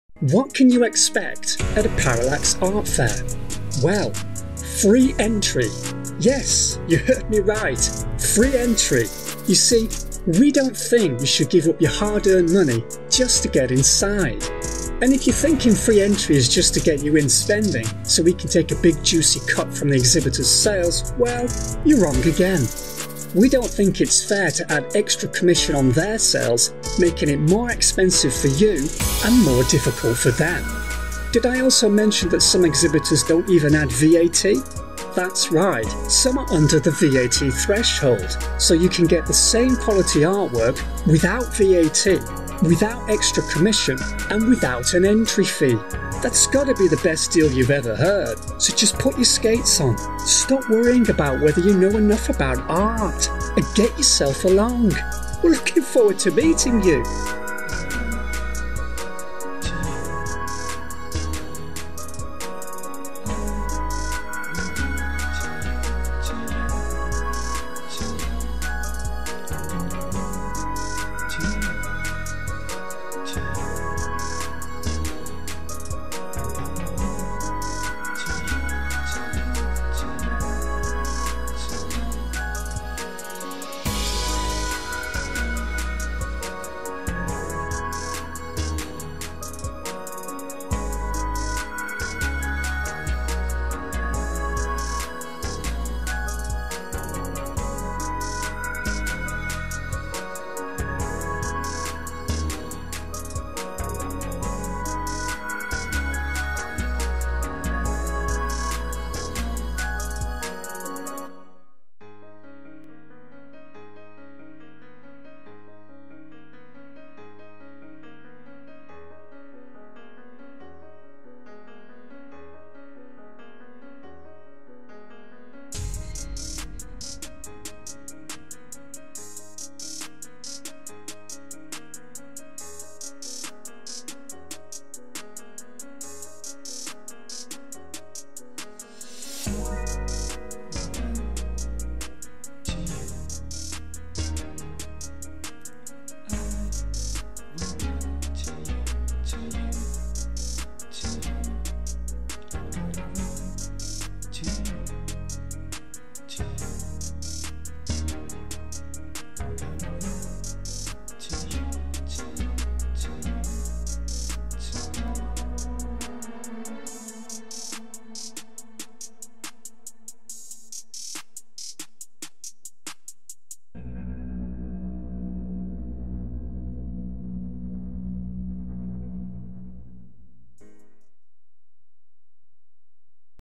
Parallax Art Fair (London, UK)
Parallax Art Fair: Europe's largest wholesale fair for art and design. 30+ editions over 10 years.

300+ independent exhibitors from around the world display thousands of handmade products. with 0% commission and, often, without VAT. Tickets are free.

Don't pay more for the same quality artists. Go to them direct!

VENUE: Kensington Town Hall, Royal Borough of Kensington and Chelsea, London,

Video Production: Impact
Editorial Production: PAF
Music copyright: C G Barlow